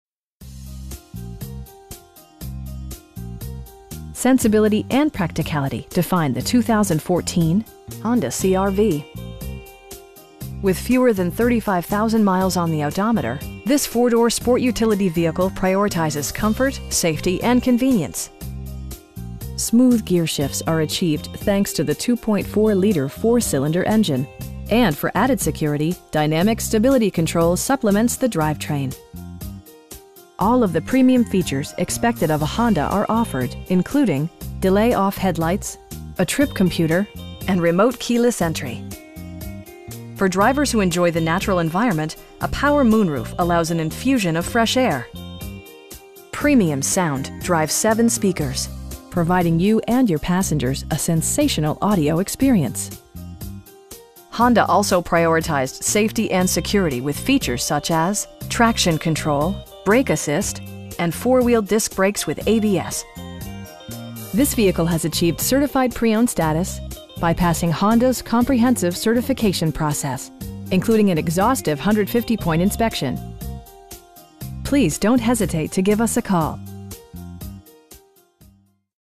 2014 Honda CR-V EX-L FWD in Orange City, FL 32763
Sensibility and practicality define the 2014 Honda CR-V. With fewer than 35,000 miles on the odometer, this 4 door sport utility vehicle prioritizes comfort, safety and convenience. Smooth gearshifts are achieved thanks to the 2.4 liter 4 cylinder engine, and for added security, dynamic Stability Control supplements the drivetrain. All of the premium features expected of a Honda are offered, including: delay-off headlights, a trip computer, tilt steering wheel, power door mirrors and heated door mirrors, cruise control, an overhead console, and remote keyless entry. Features such as automatic climate control and leather upholstery prove that economical transportation does not need to be sparsely equipped. For drivers who enjoy the natural environment, a power moon roof allows an infusion of fresh air. Premium sound drives 7 speakers, providing you and your passengers a sensational audio experience. Honda also prioritized safety and security with features such as: head curtain airbags, front side impact airbags, traction control, brake assist, a security system, and 4 wheel disc brakes with ABS. This vehicle has achieved Certified Pre-Owned status, by passing Honda's comprehensive certification process, including an exhaustive 150-point inspection! We have the vehicle you've been searching for at a price you can afford. Please don't hesitate to give us a call.